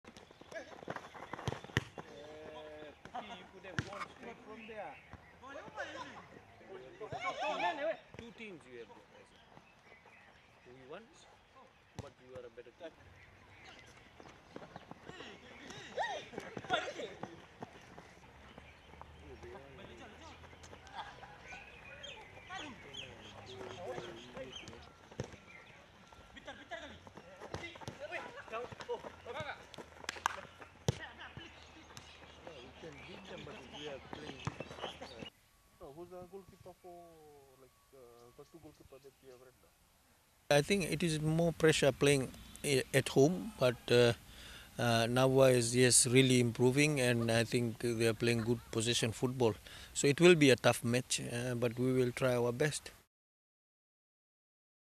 Nadi Shifts Focus To VPL
The Nadi Football team wants to continue their Vodafone Premier League game on a winning note when they take on Navua this Sunday at Prince Charles Park.